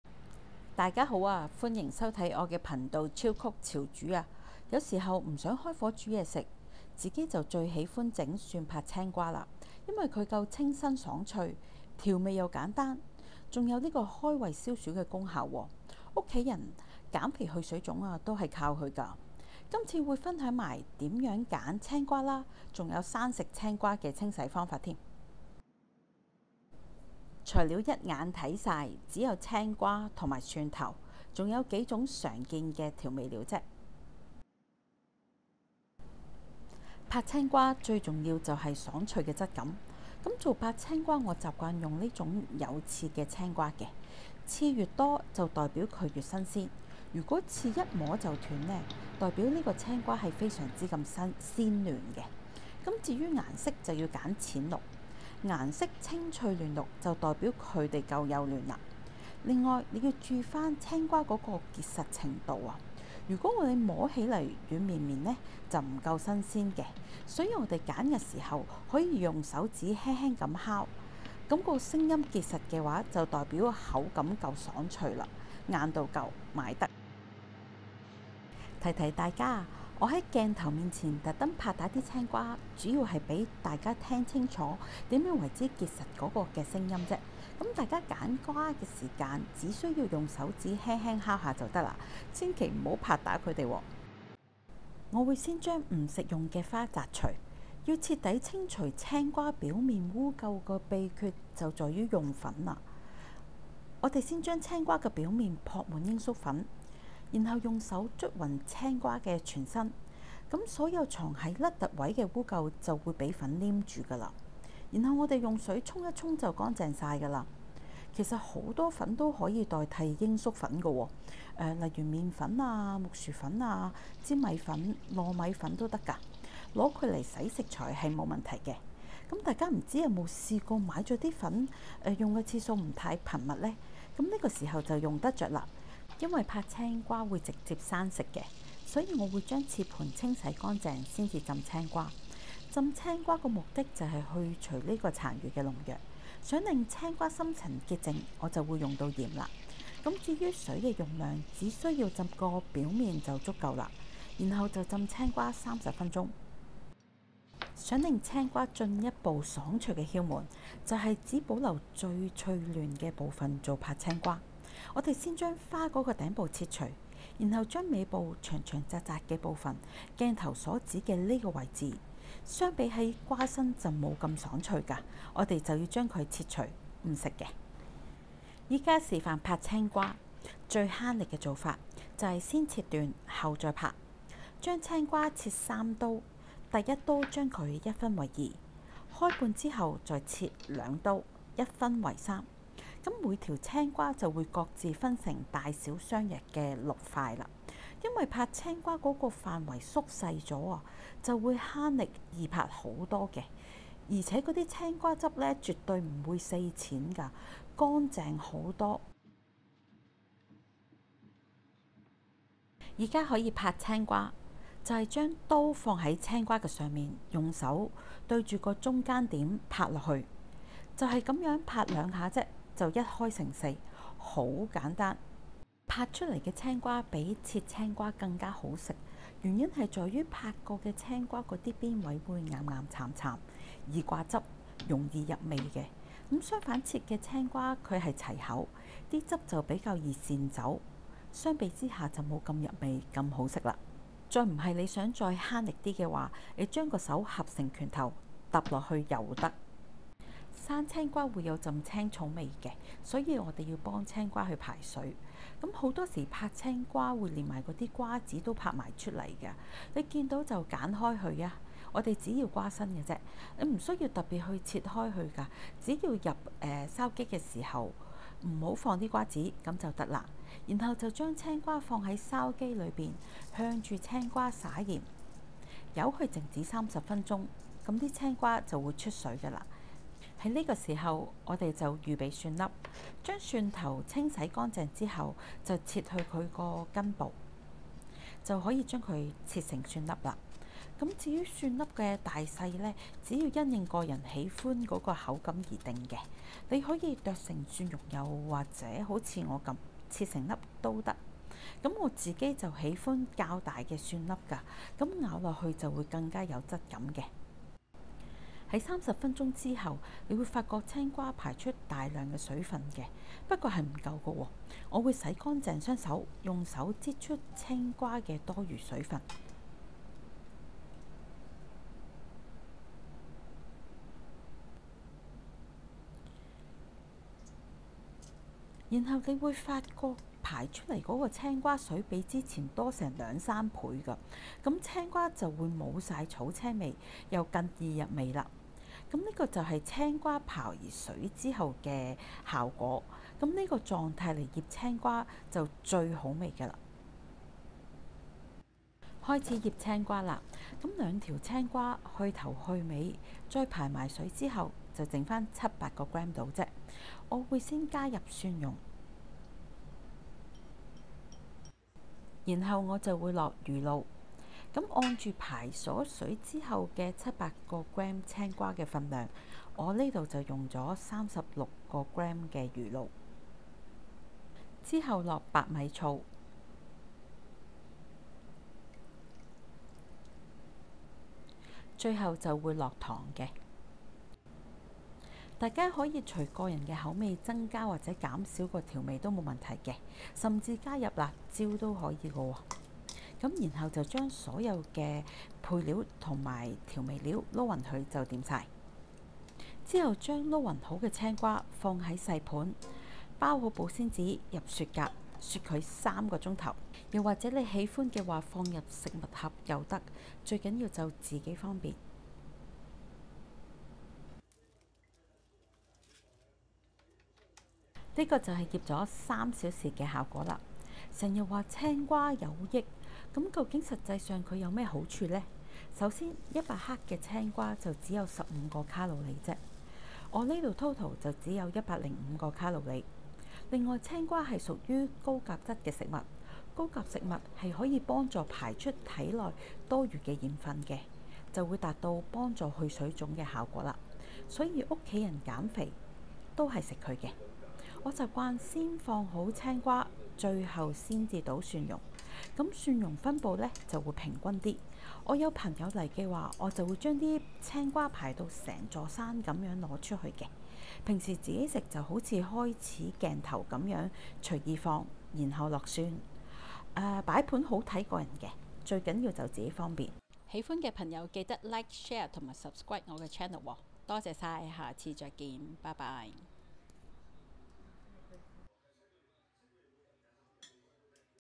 拍青瓜Smacked Cucumber～無火煮食必學簡單食譜：分享入味、爽、脆、嫩做法《內含揀瓜、洗瓜，拍瓜技巧》❗️低卡涼伴菜《105卡路里😱》減肥食佢掂曬‼️
拍青瓜-必學簡單食譜：分享入味、爽、脆、嫩揀瓜和做法❗️低卡涼伴菜《只有105卡路里😱》減肥食佢掂曬‼️

拍青瓜：
青瓜：850g (排水後700g)
鹽:5g. (排水用）

醃料：
蒜粒：50g
魚露 36 g
醋：22 g
糖：18g